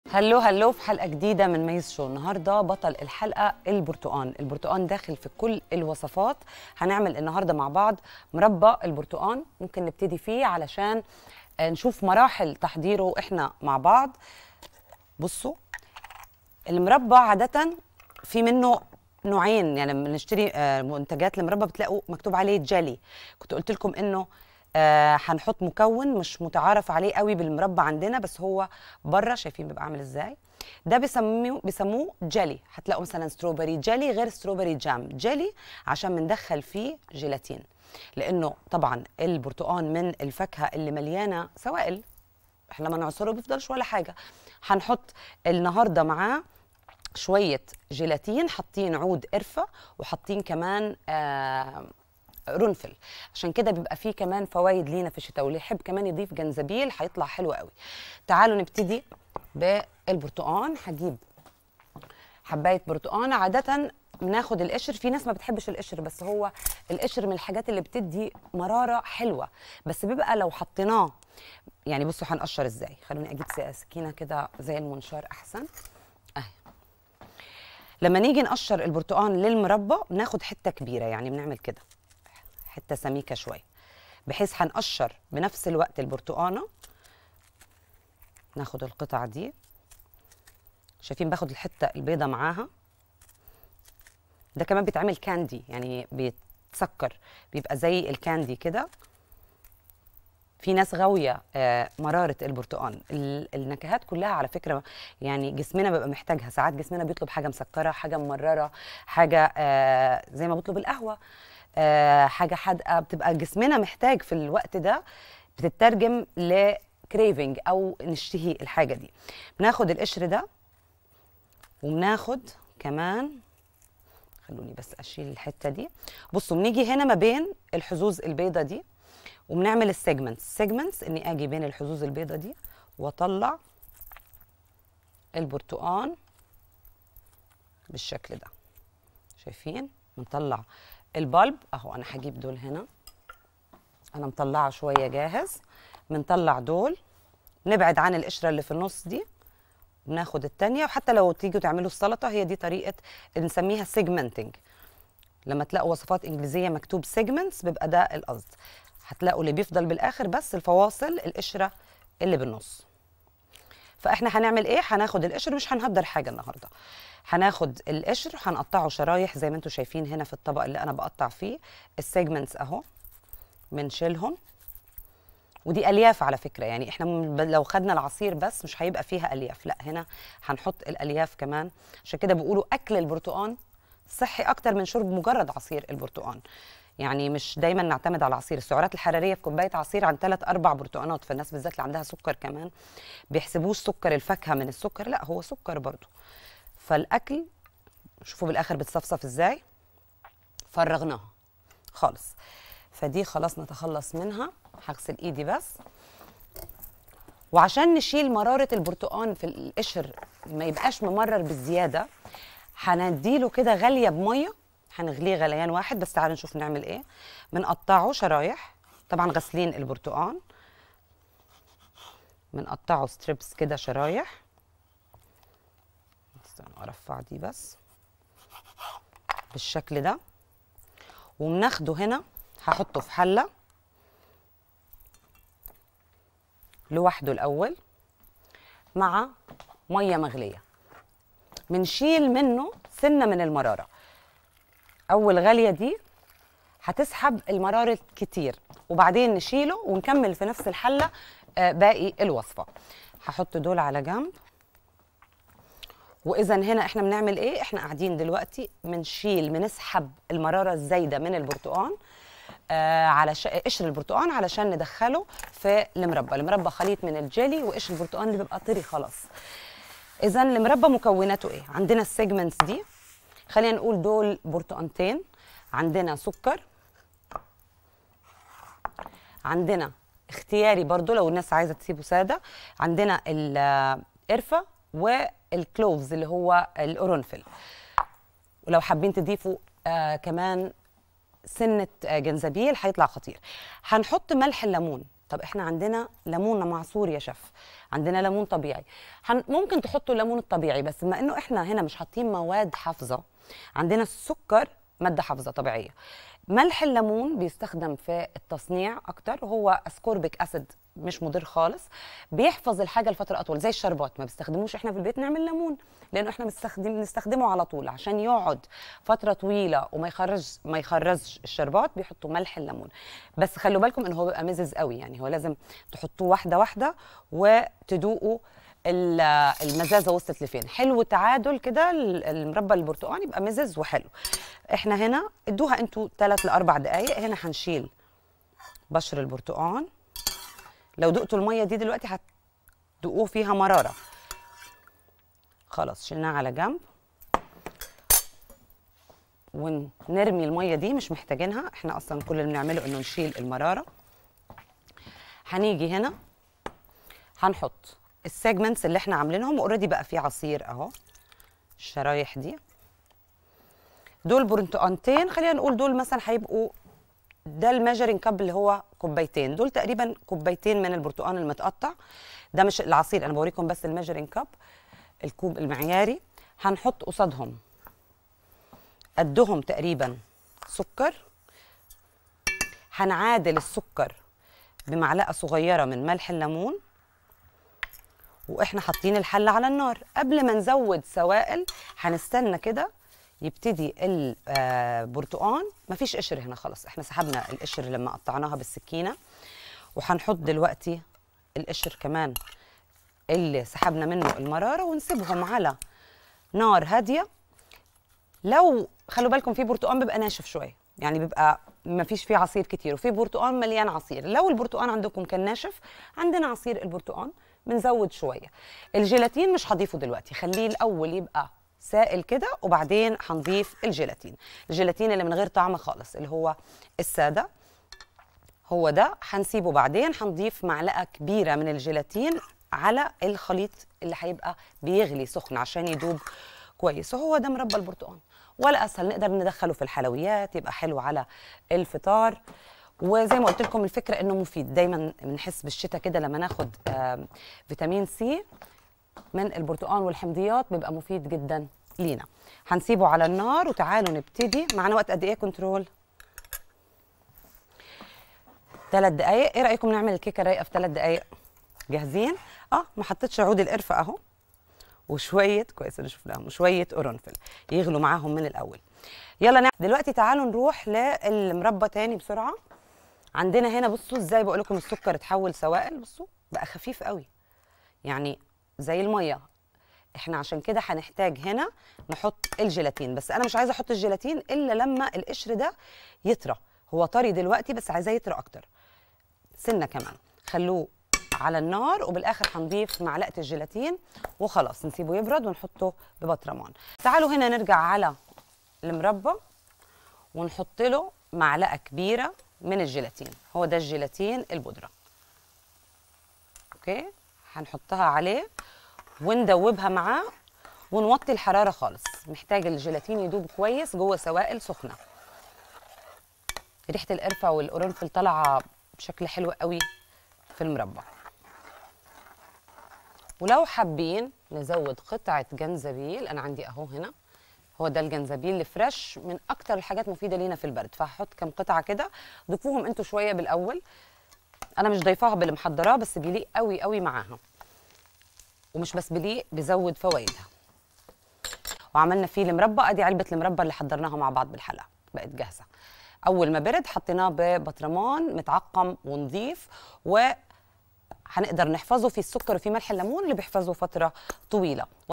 خطوة بخطوة ازاي تعملي مربى البرتقال وتطلع معاكي مظبوطة وكل الاسرار اللي تخليها ألذ من الجاهزة😋🍊
كيكة برتقال:
علبة لبن مكثف محلى
١ كوب دقيق
١/٢ كوب زيت او زبدة سايحة
٣ بيضات كبار
١/٢ ١ معلقة صغيرة بيكنج صودا
بشر برتقال
رشة ملح
للآيسنج:
سكر بودرة
عصير برتقال

مربى برتقال:
برتقال
سكر
جيلاتين
عود قرفة
قرنفل
ملح ليمون
جنزبيل طازه

بط بصوص البرتقال:
بط مسلوق
بصل
ثوم
بشر برتقال
جوزة الطيب
قرنفل
روز ماري
عصير برتقال
صويا صوص

بطاطس بيوريه:
بطاطس مسلوقة
بطاطا مسلوقة
زبدة
كريمة
ملح

عصير برتقال:
عصير برتقال
عصير ليمون
شرائح برتقال
شرائح ليمون
عود قرفة
قرنفل
جنزبيل
عسل أبيض
فلفل اسود